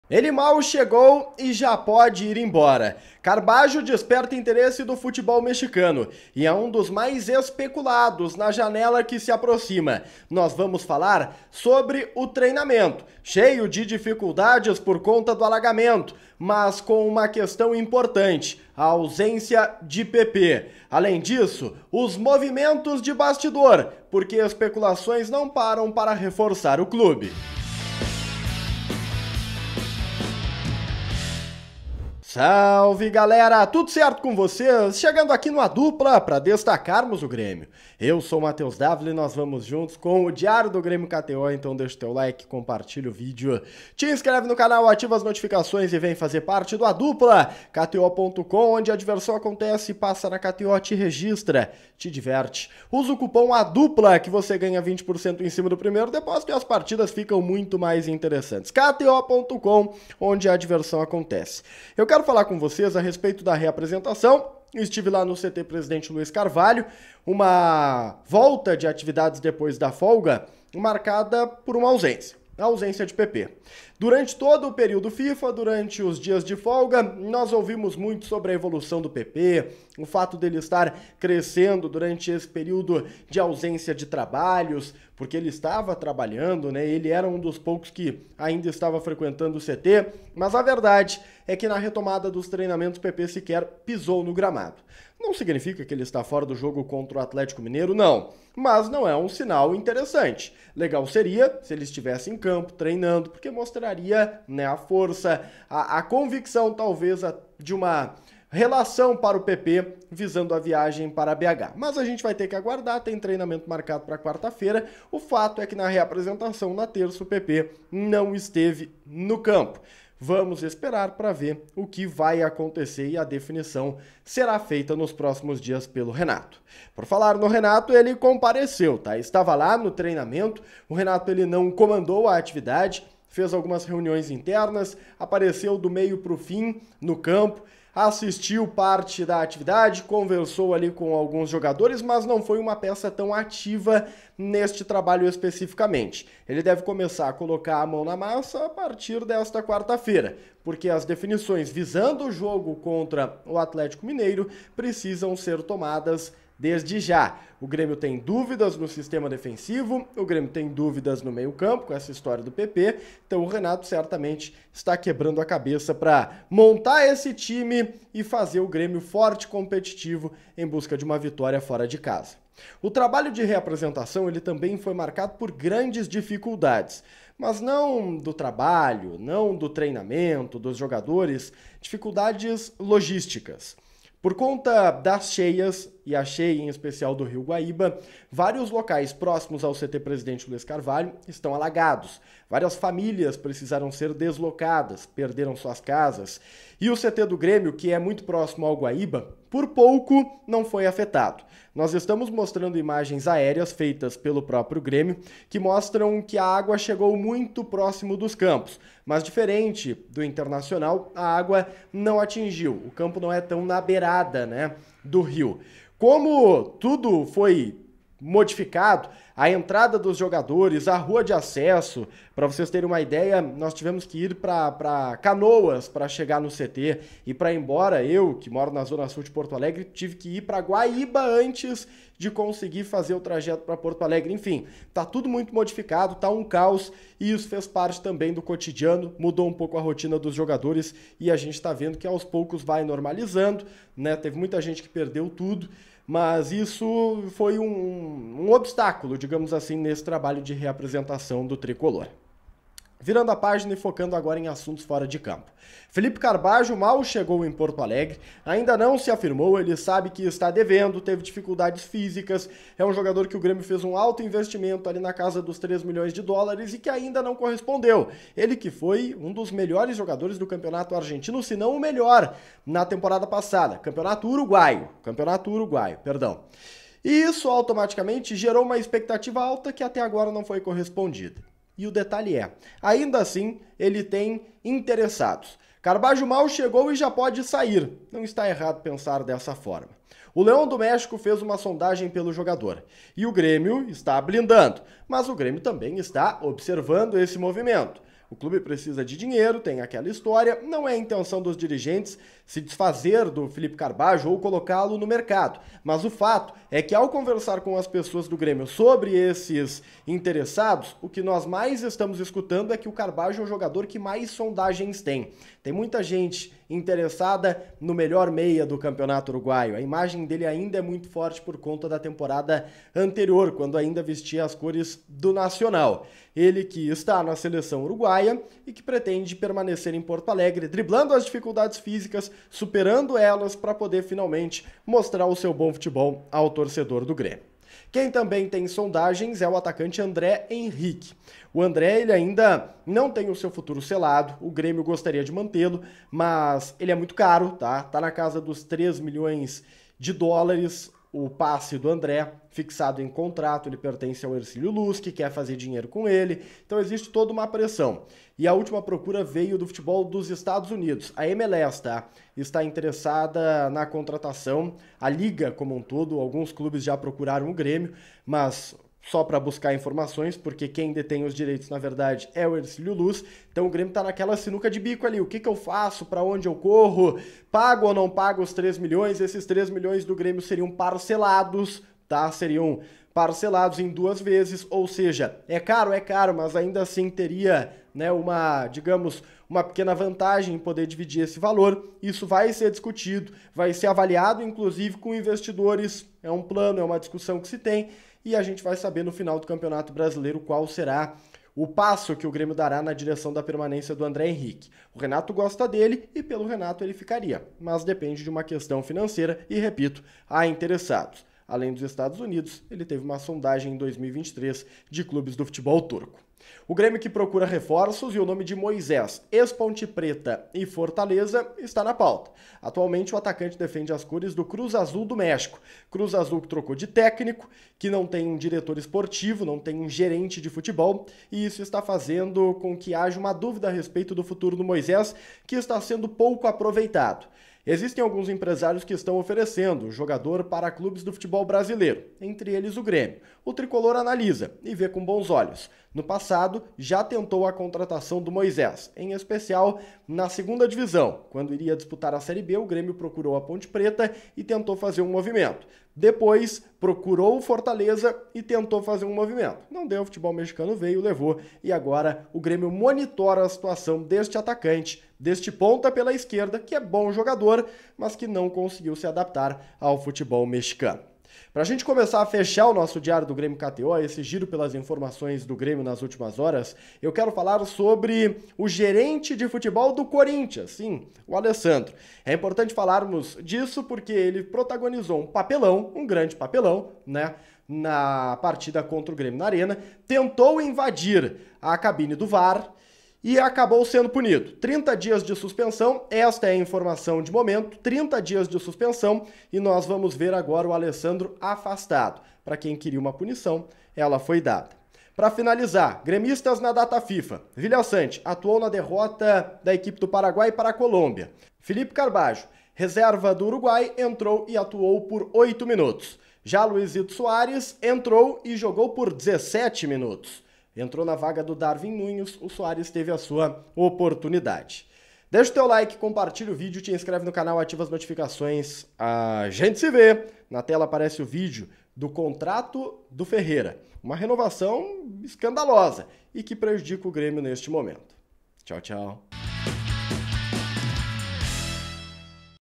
🔵⚫ Diário do Grêmio KTO: Sondagens aumentam por Carballo | Volta aos treinos | Moisés no alvo
Segue o IG e o TikTok do ADUPLA: @ADuplaYT